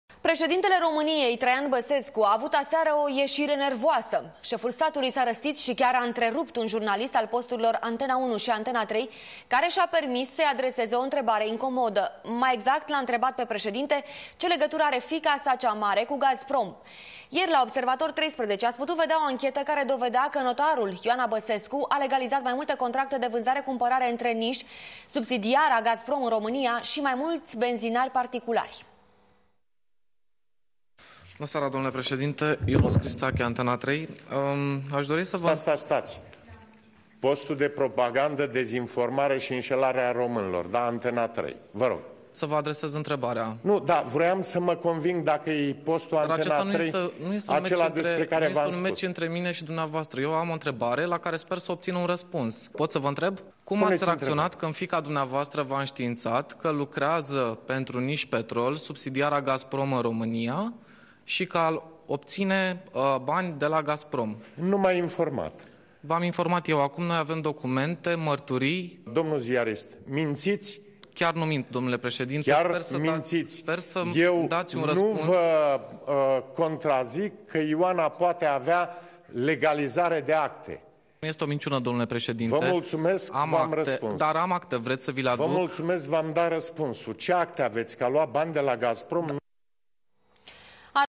Preşedintele şi-a ieşit din sărite
Preşedintele României, Traian Băsescu a avut aseară o ieşire nervoasă. Şeful statului s-a răstit şi chiar a întrerupt un jurnalist al posturilor Antena 1 şi Antena 3 care şi-a permis să-i adreseze o întrebare incomodă., Stiri, Antena 1, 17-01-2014 Abonează-te la canal